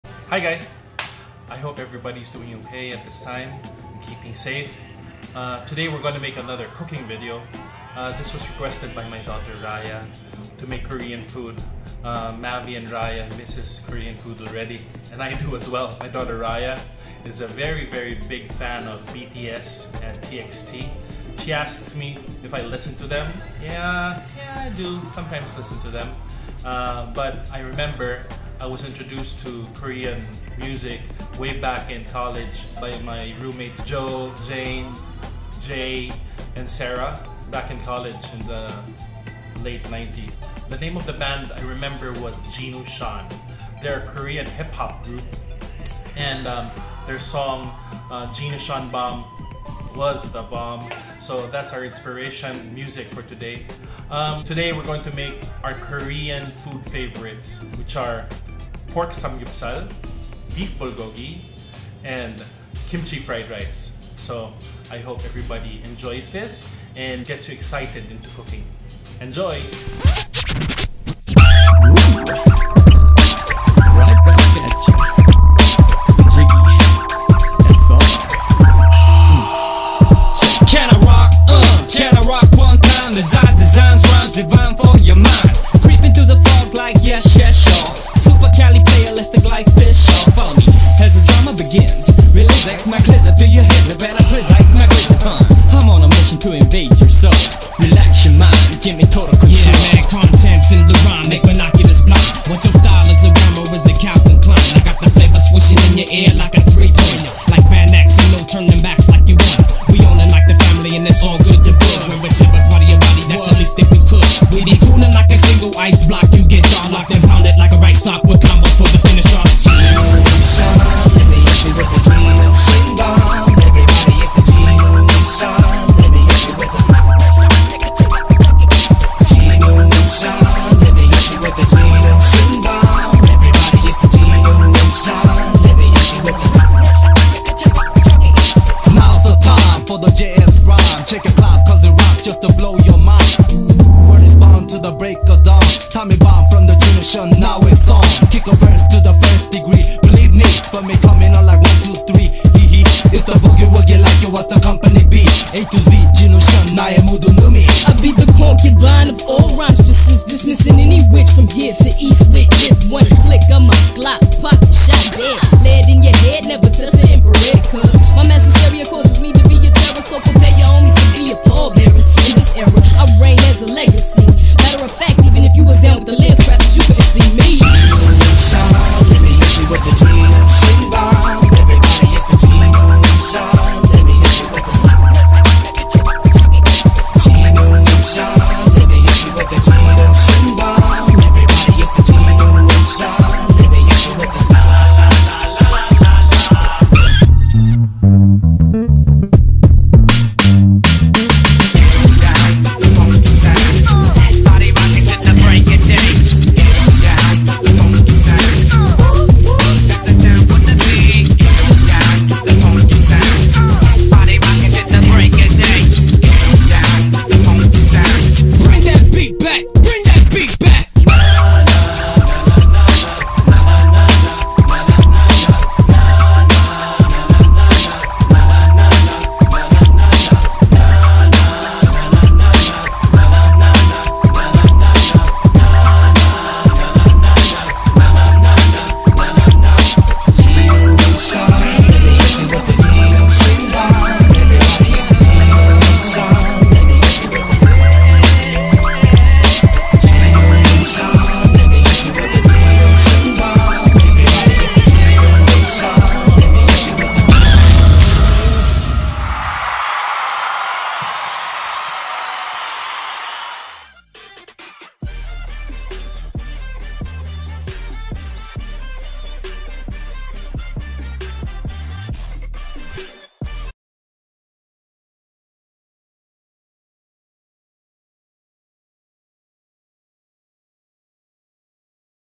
Quarantine Style Kimchi Fried Rice, Beef Bulgogi, and Pork Samgyeopsal
INGREDIENTS FOR KIMCHI FRIED RICE

- 1/4 CUP LEEKS -SLICED
- 3 TBSP. CANOLA OIL
- 1 TBSP. SAMJANG
- 3 TBSP. SOY SAUCE
- 2 TBSP. GOCHUJANG
- 1/2 TBSP. SESAME OIL
- 1 CUP KIMCHI- CHOPPED
- 4 CUPS RICE ( BETTER IF IT'S A DAY OLD RICE)
- 3 EGGS - BEATEN

INGREDIENTS FOR THE BEEF BULGOGI:

- 500 G. SUKIYAKI CUT BEEF
- 1 TBSP. GINGER
- 1/2 GREEN APPLE
- 1/4 CUP LEEKS - SLICED THINLY
- 1/4 CUP SOY SAUCE
- 1 TBSP. SESAME SEEDS - SLIGHTLY TOASTED
- 2 TBSP. CANOLA OIL.
- 1 TBSP BROWN SUGAR
- 3 CLOVES GARLIC PEELED

INGREDIENTS FOR PORK SAMGYUPSAL:

- 1/2 TSP. AROMAT OR GINISA FLAVOR MIX
- 500 G. PORK BELLY SKINLESS AND BONELESS, CUT INTO 1 CM SLICES
- 1/4 CUP BEER OR SODA WATER
- 2 PINCH SALT
- 1 PINCH PEPPER
- 1 TBSP CANOLA OIL
- 1/2 TBSP. SESAME OIL